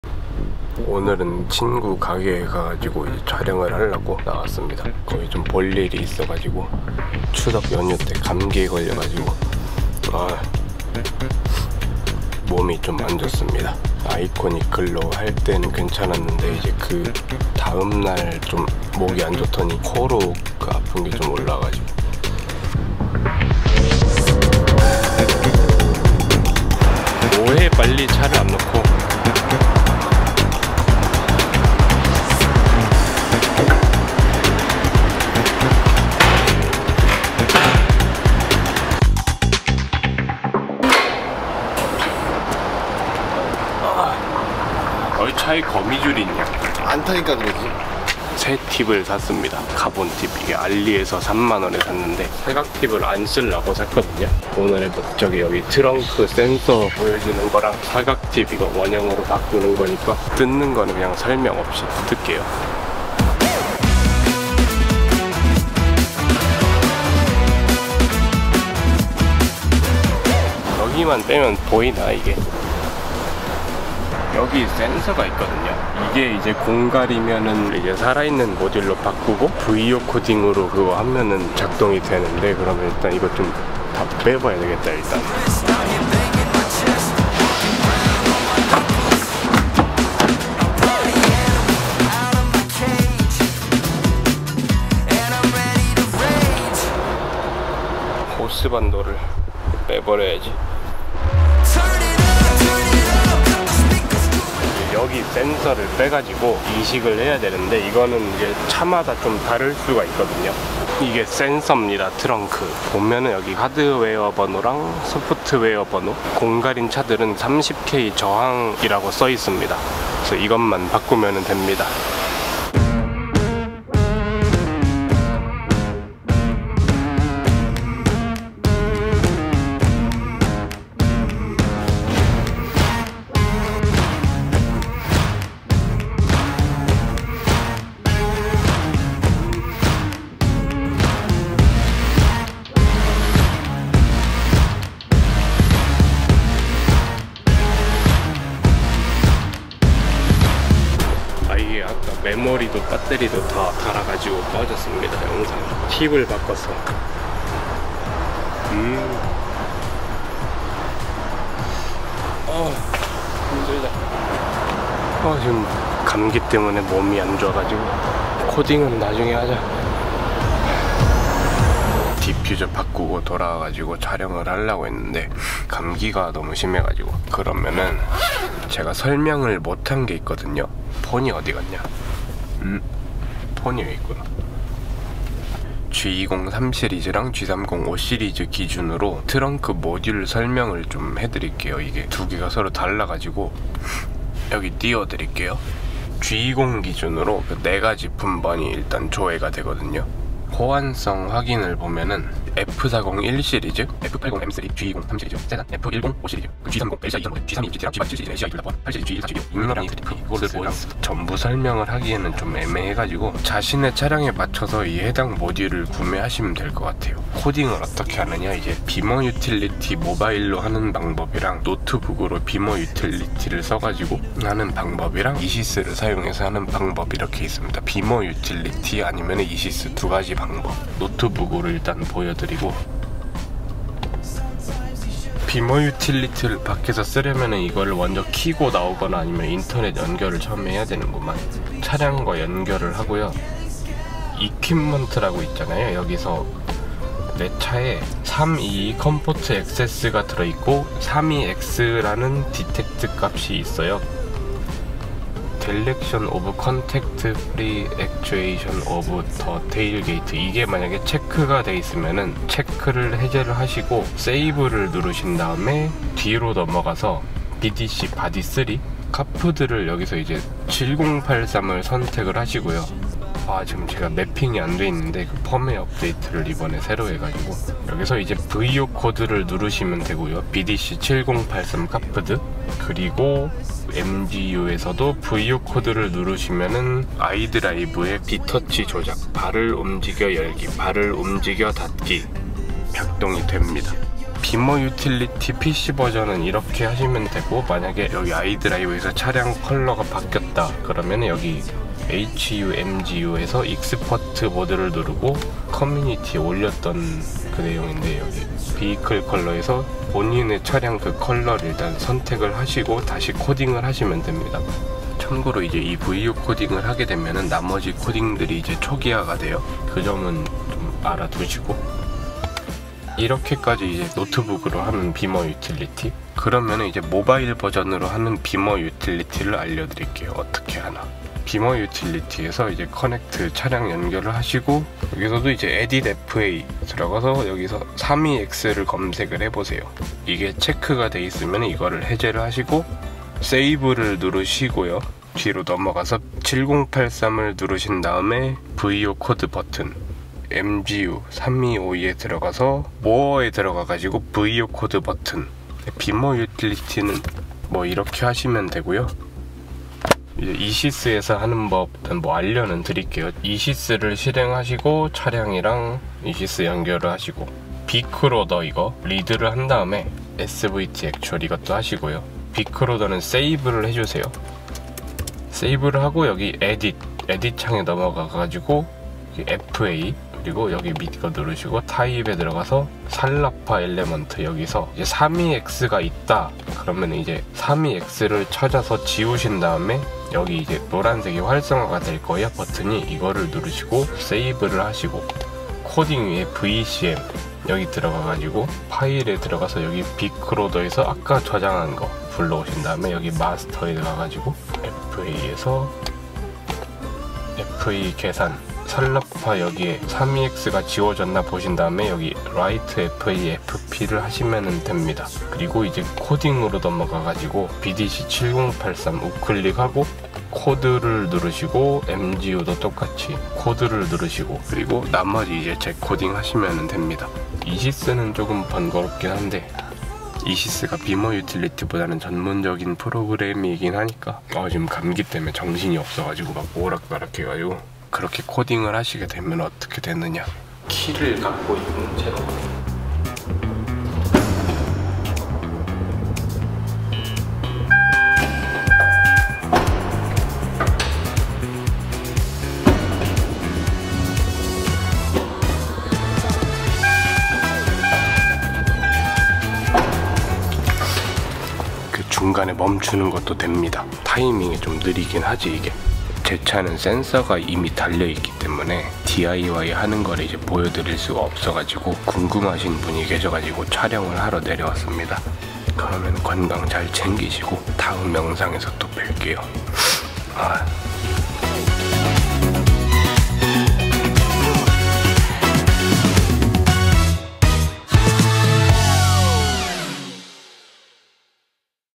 반도체 대란으로 옵션이 사라진 차량들을 위한 (BMW 트렁크 킥모션 활성화 방법)
원활한 영상 제작을 위하여 멤버십 가입을 해주심이...?

공갈을 넣어줄 정성이면 그냥 모듈을 넣어주지...

촬영장비
Sony A6400

0:00 인트로
0:27 어딘가 도착
0:48 오늘의 업무
1:03 320i로 컨버전
1:44 킥모션 센서 모듈
2:01 다시 시작
2:31 컨버전 끝
2:57 킥모션 센서 모듈 품번
3:37 킥모션 코딩의 방법들
4:01 비머 유틸리티 PC로 하는 방법
5:31 비머 유틸리티 모바일로 하는 방법
6:02 Esys로 하는 방법
7:17 킥모션 작동 방법
7:45 아웃트로